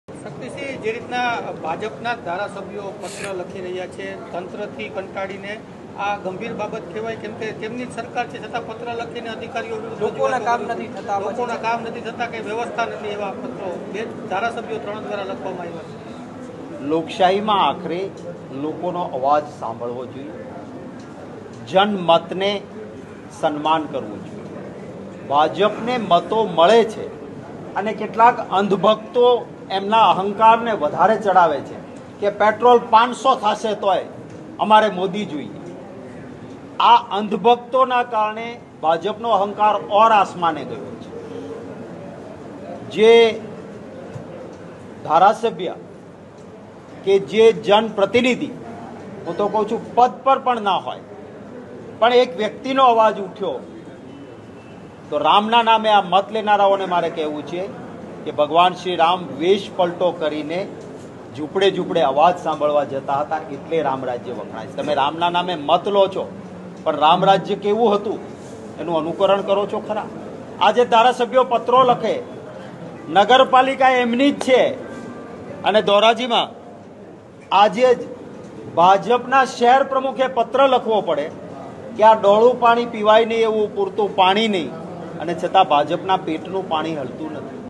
લોકશાહીમાં લોકોનો અવાજ સાંભળવો જોઈએ જનમતનું સન્માન કરવું જોઈએ
લોકશાહીમાં લોકોનો અવાજ સાંભળવો જોઈએ જનમતનું સન્માન કરવું જોઈએ. ભાજપ અહંકારમાં શાસન કરી રહી છે.

ડહોળું પાણી કે જે પીવા લાયક નથી તેની રજૂઆત ભાજપના જ નેતા કરે છતાં ભાજપ સરકારનાં પેટનું પાણી હલતું નથી,